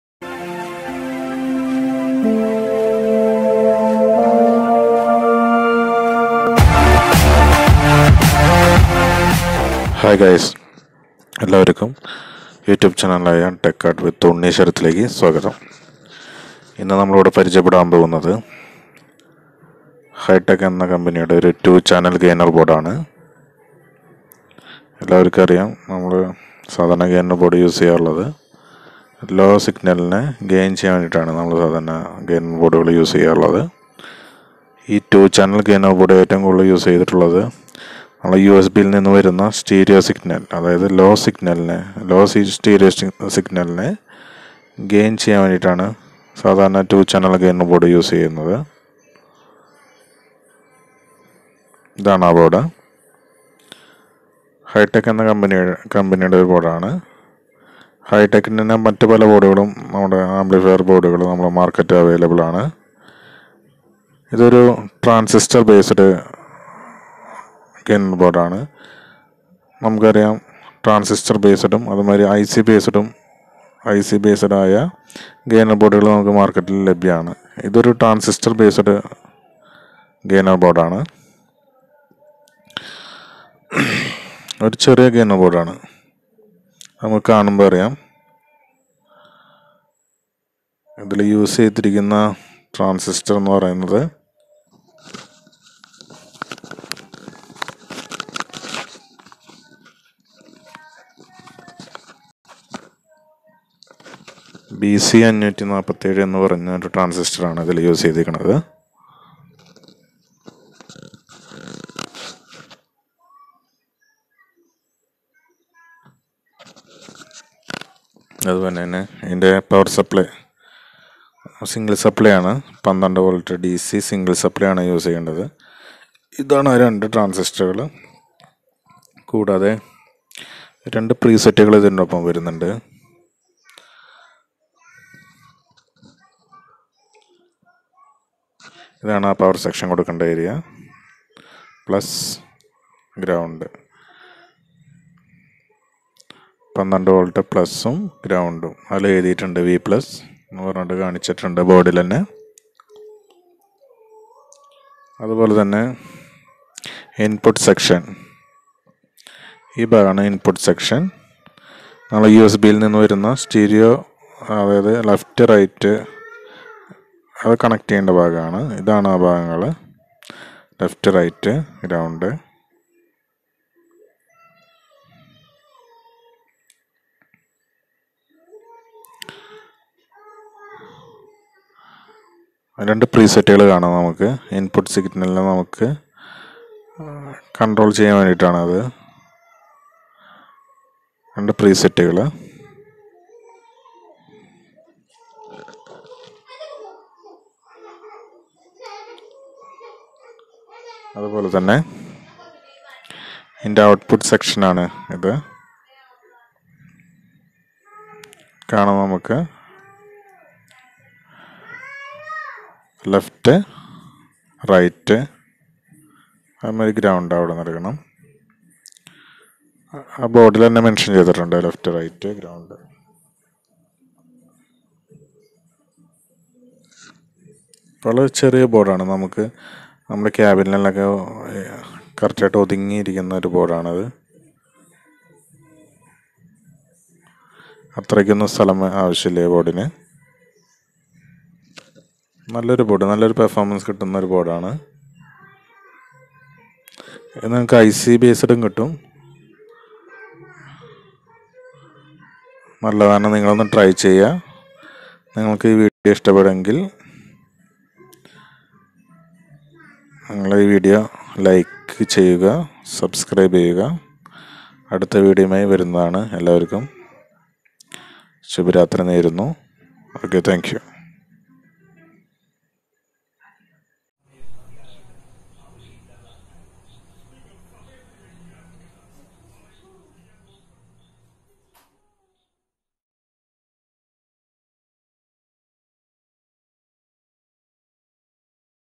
2 CHANNEL GAINER BOARD
This is 2 channel gainer board. This board mainly used for gaining stereo signals of USB board in an amp. Explained. In Malayalam